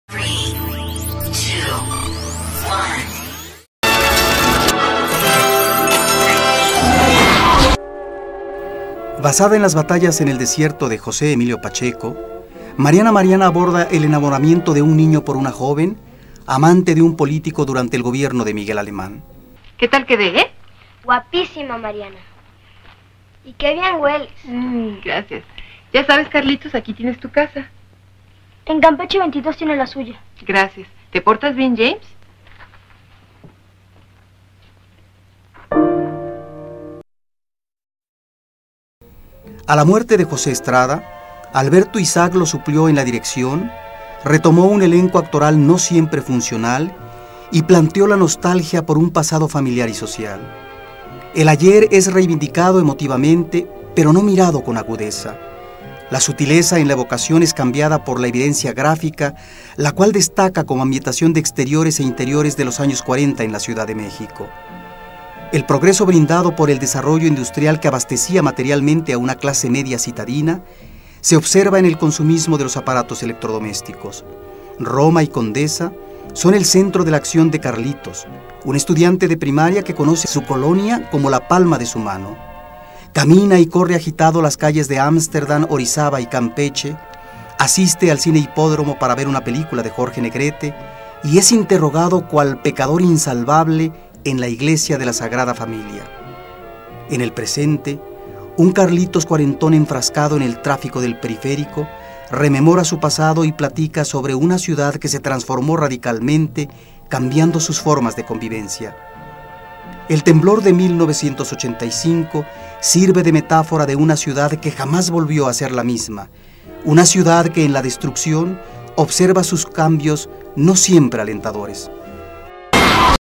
Mariana, Mariana (1987)
En la Sección "México de mis Recuerdos" platicamos de la película "Mariana, Mariana" (1987) de Alberto Isaac. Guión de Vicente Leñero, basado en la novela "Las Batallas en el Desierto" de José Emilio Pacheco.